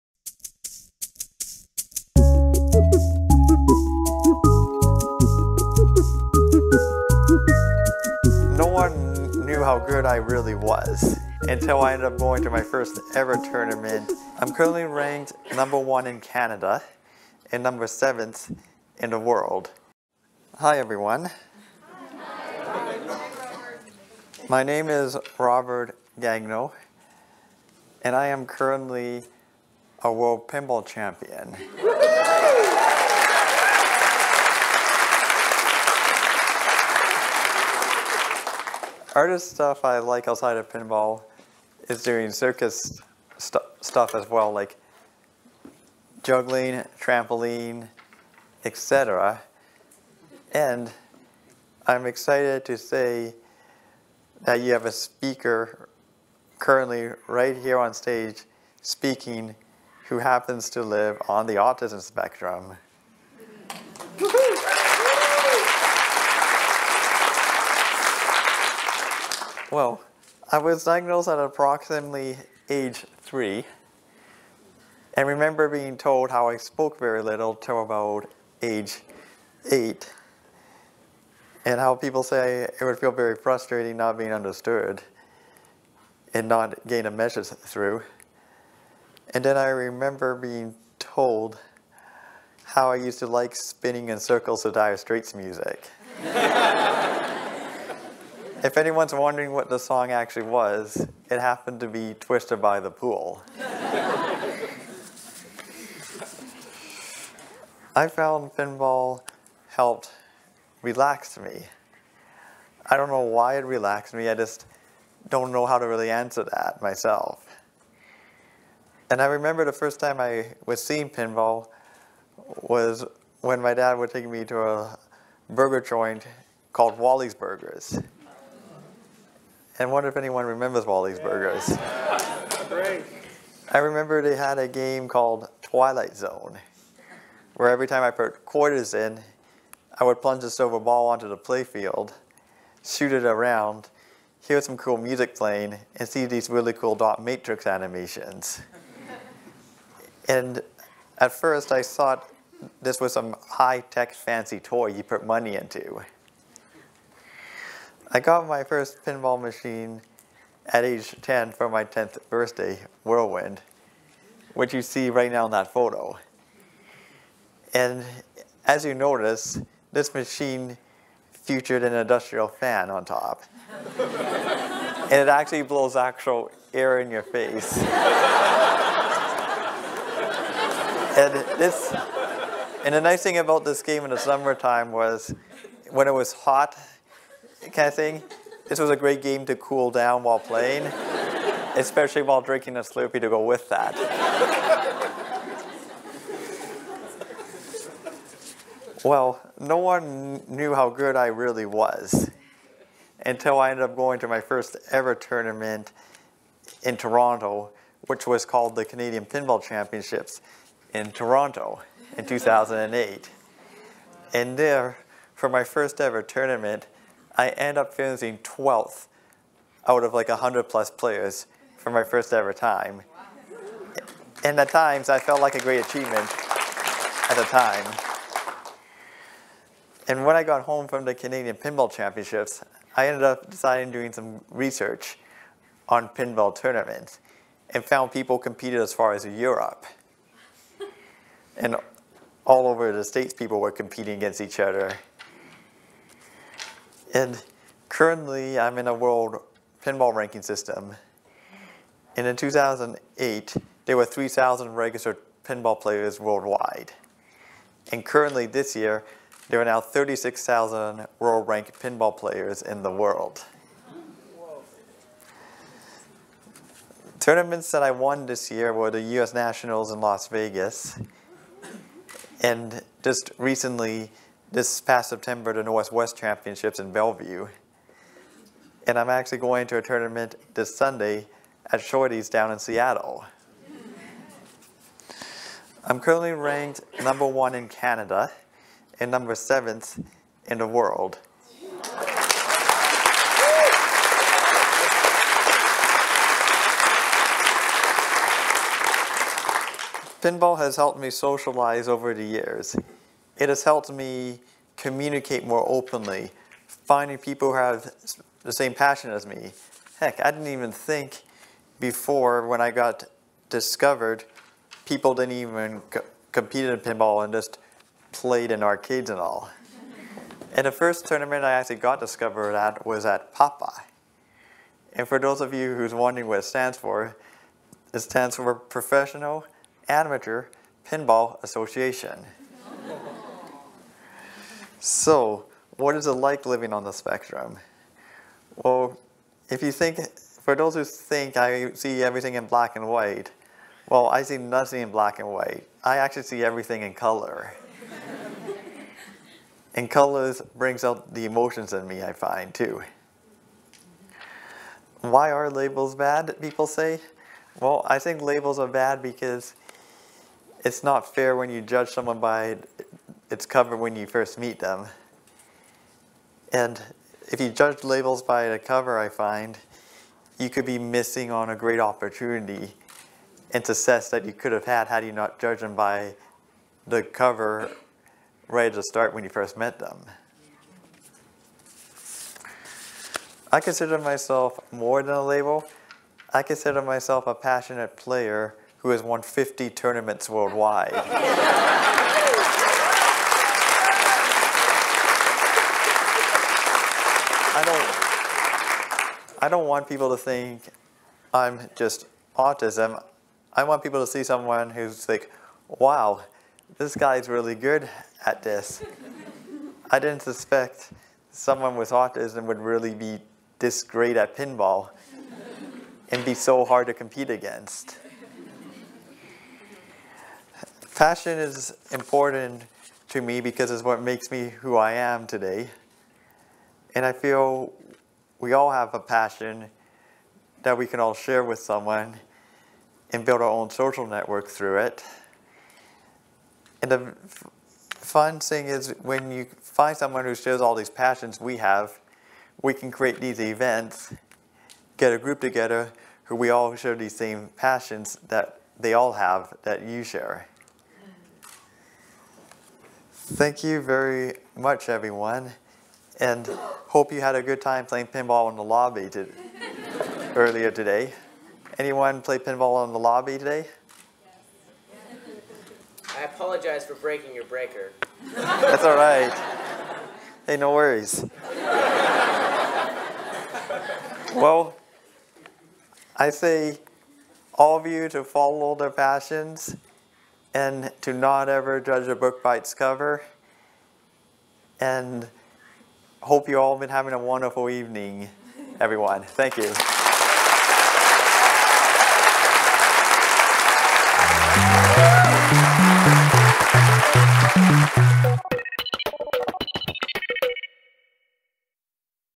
On stage at Interesting Vancouver
I was honored to be asked to provide a presentation at Interesting Vancouver 2015